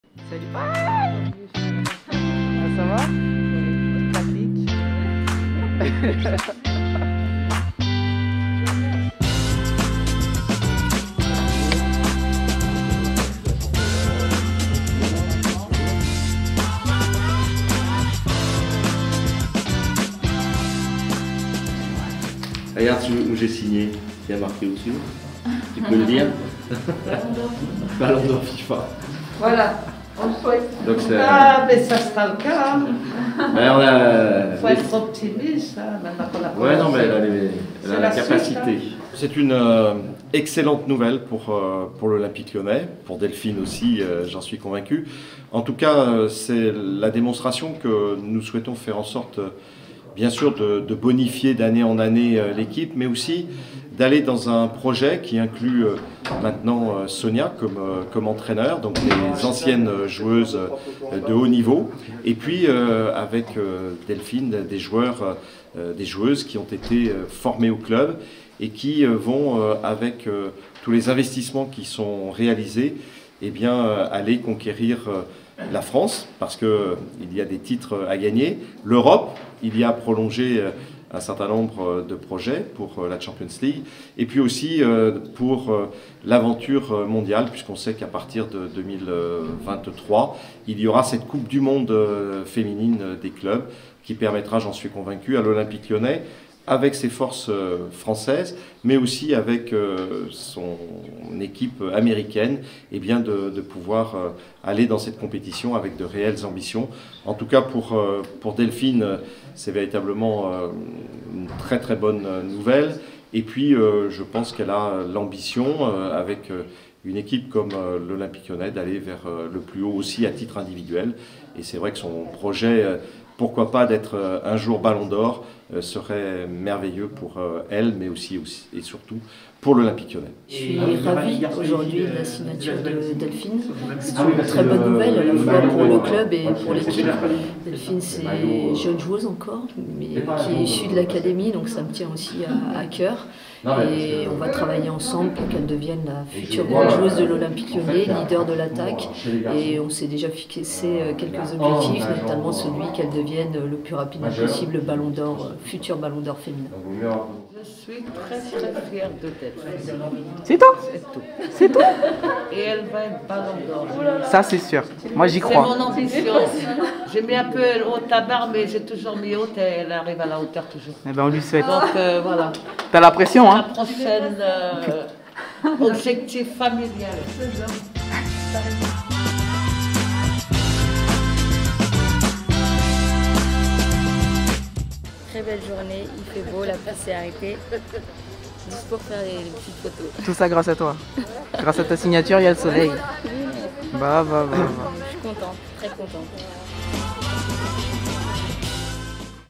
Prolongation de contrat de Delphine Cascarino | Olympique Lyonnais
Retour en images sur la prolongation de contrat de Delphine Cascarino avec l'Olympique Lyonnais jusqu'en 2024 !